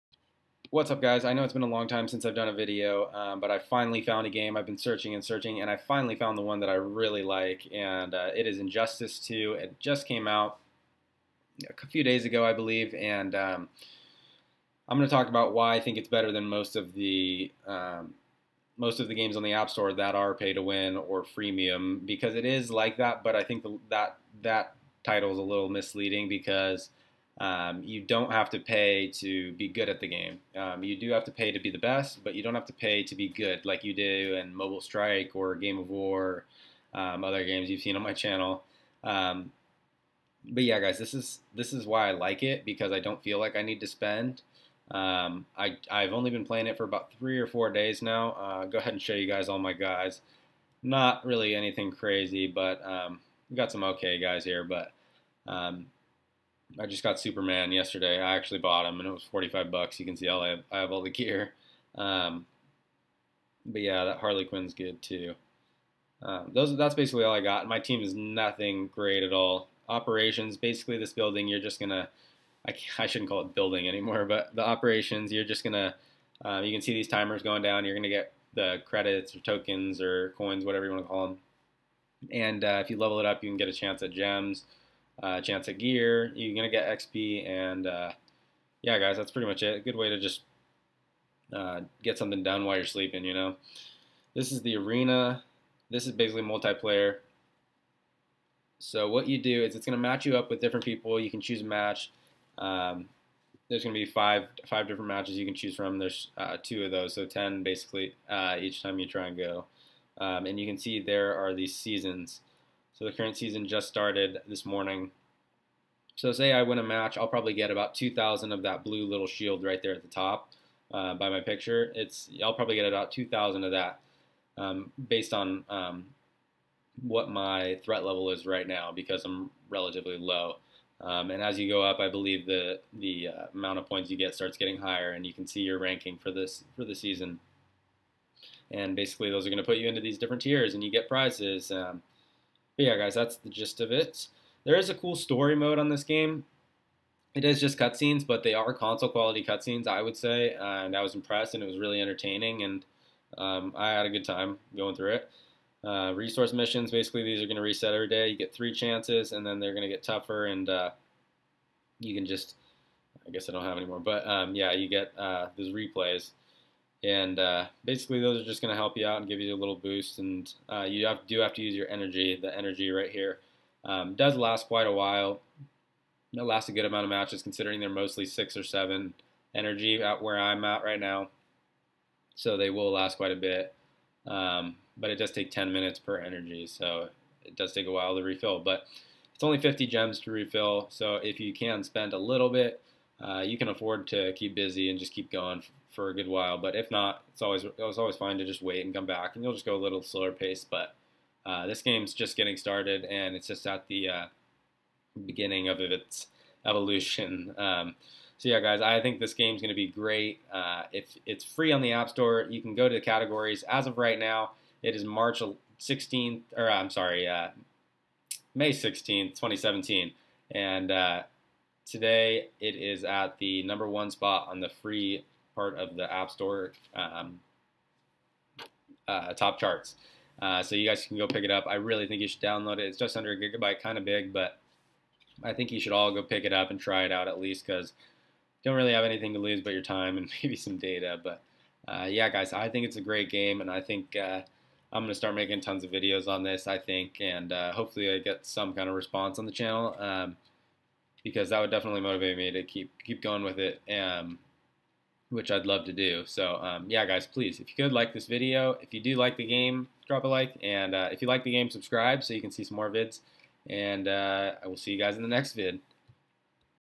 Injustice 2 Mobile - Why I LOVE It
My little review/thoughts on the new Injustice 2 game for iOS & Android devices. Super excited for more updates on this one!!